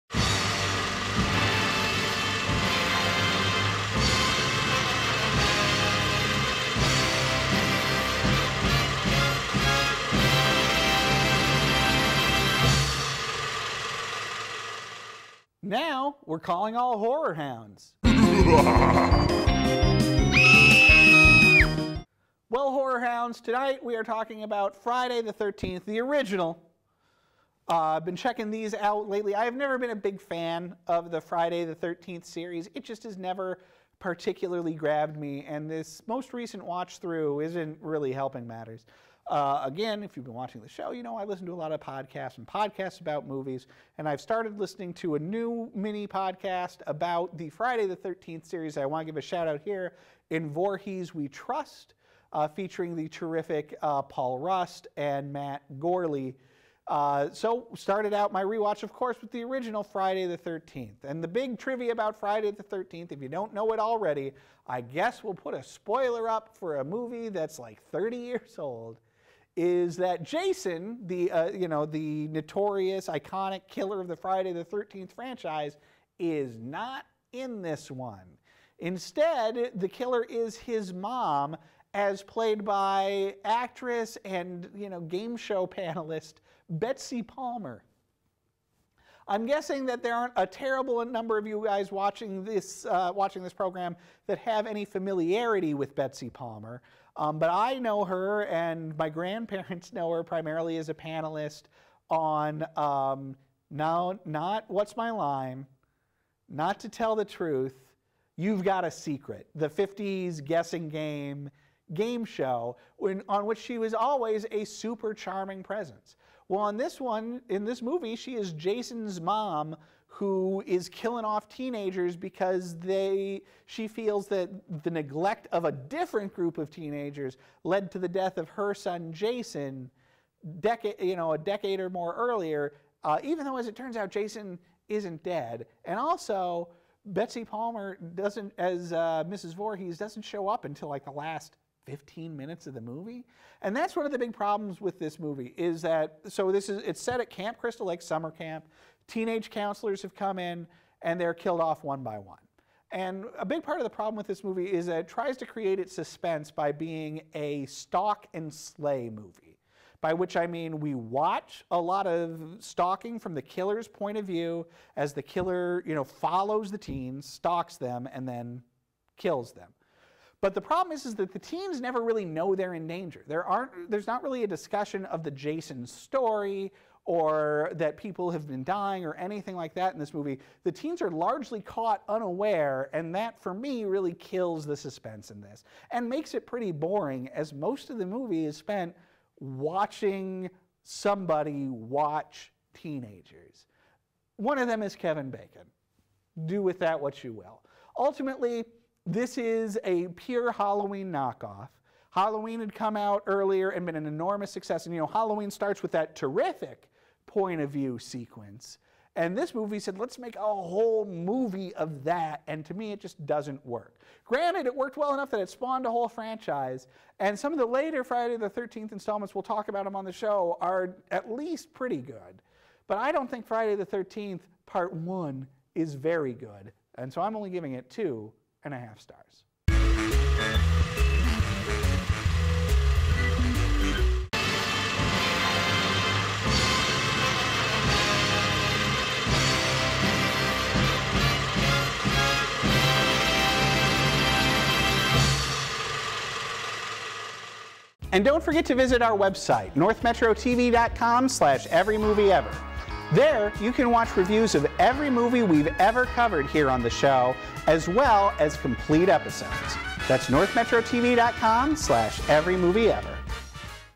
Every Movie Ever - Friday the 13th (1980)
We're reviewing every movie ever made. Today, Friday the 13th, a shallow Halloween knockoff that doesn't even feature everyone's favorite hockey masked killer.